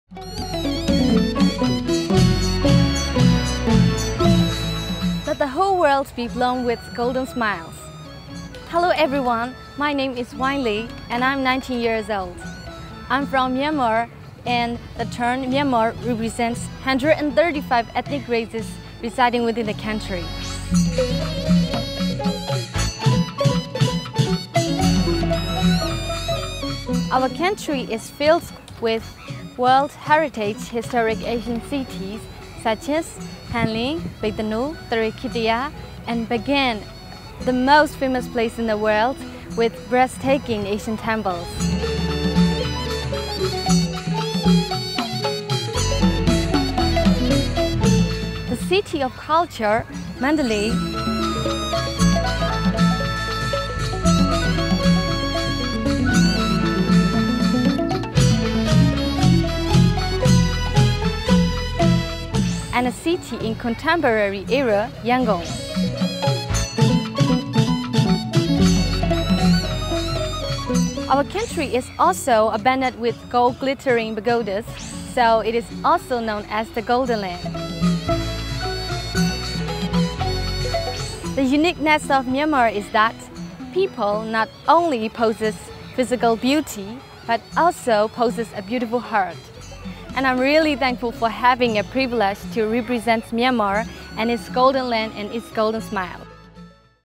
MYANMAR, Wyne Lay - Contestant Introduction: Miss World 2014
Meet Wyne Lay, Myanmar's representative for Miss World 2014!

Miss World 2014 kicks off in November, with the grand final taking place on Sunday 14th December.